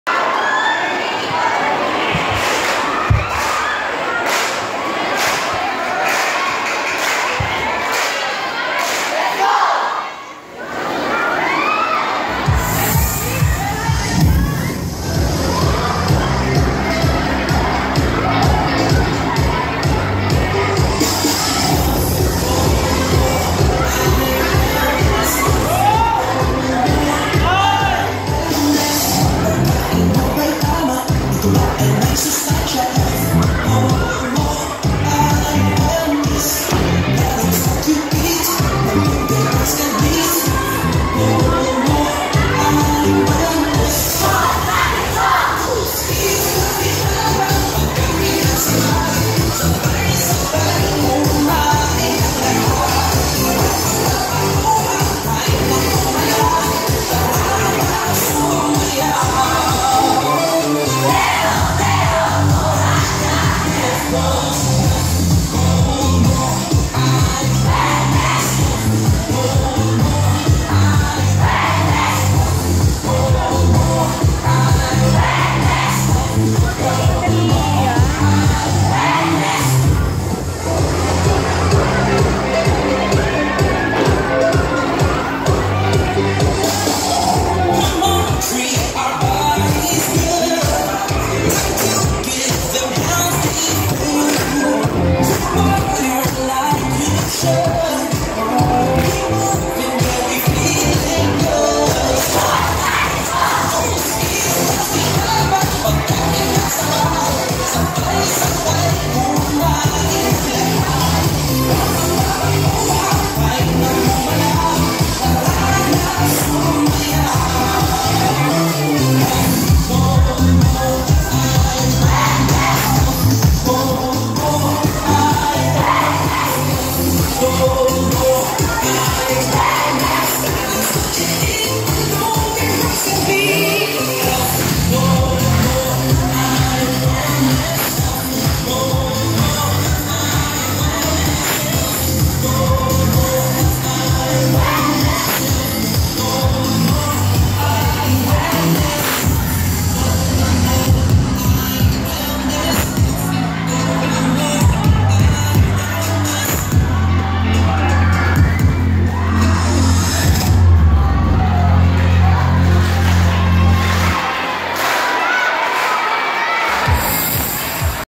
GRADE 9, 3rd Runner Up, Wellness Dance Competition 2019
Madapdap Resettlement High School celebrates Nutrition Month with an inter-grade dance competition.